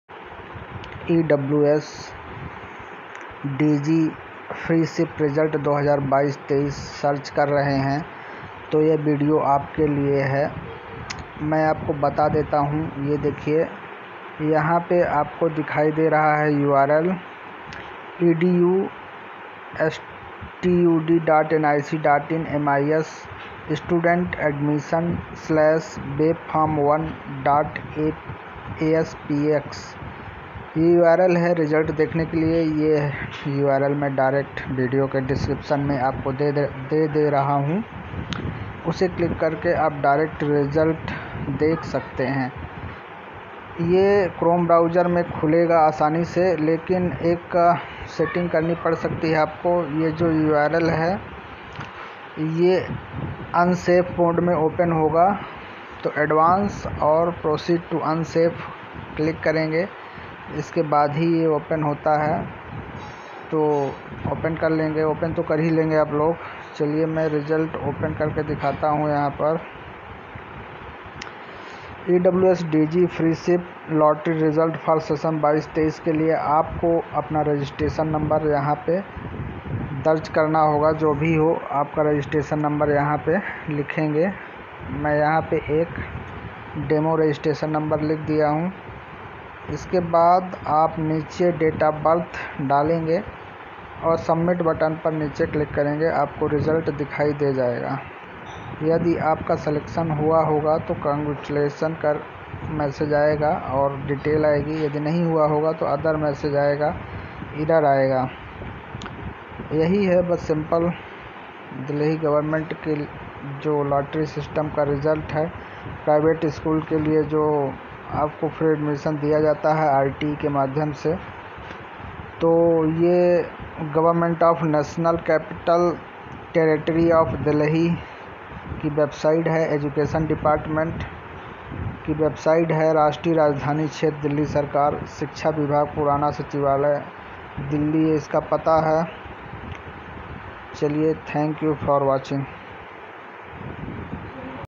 Delhi RTE Result - EWS Result 2022 23 / DG / FREESHIP Lottery Result
GOVERNMENT OF NATIONAL CAPITAL TERRITORY OF DELHI
EDUCATION DEPARTMENT, OLD SECRETARIAT, DELHI-110054
राष्ट्रीय राजधानी क्षेत्र दिल्ली सरकार
शिक्षा विभाग, पुराना सचिवालय, दिल्ली-110054
EWS/DG/FREESHIP Lottery Result For Session 2022-23